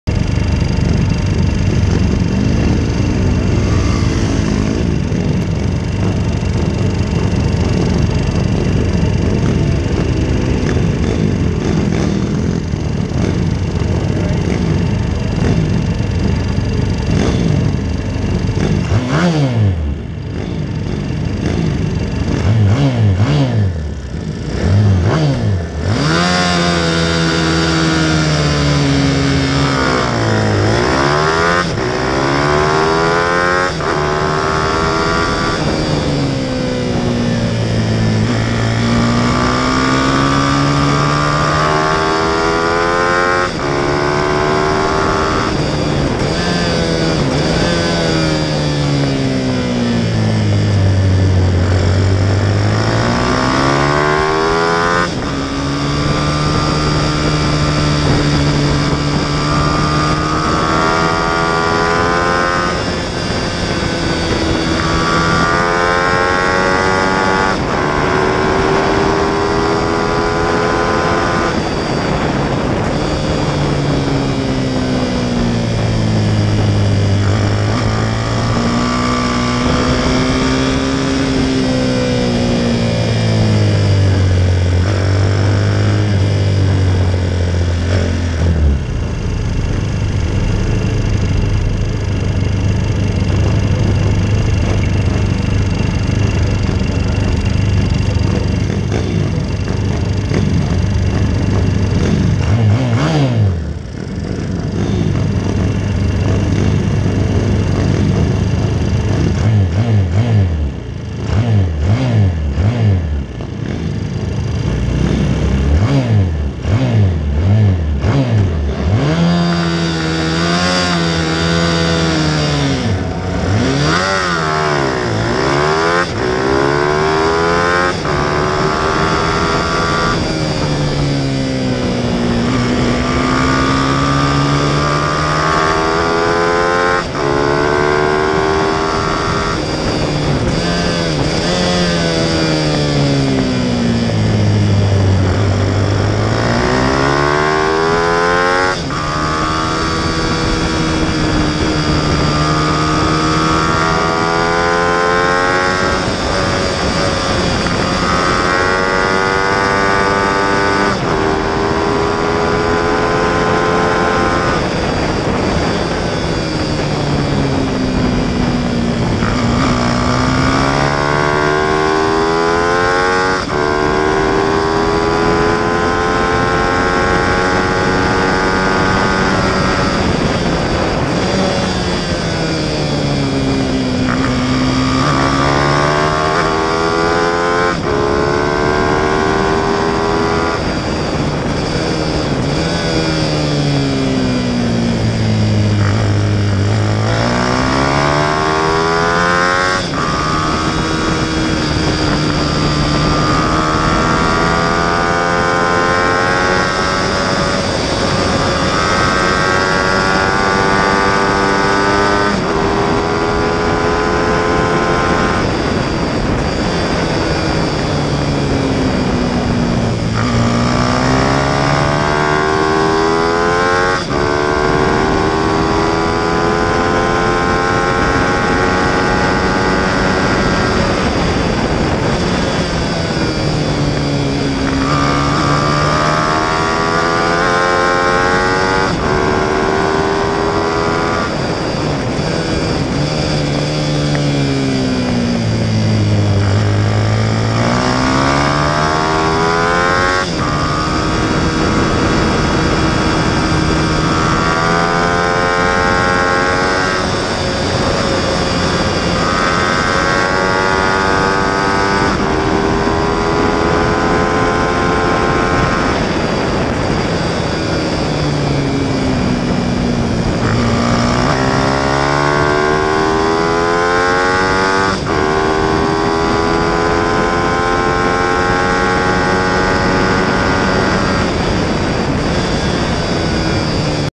FIM Round 1 Ireland
First couple of laps and sighting lap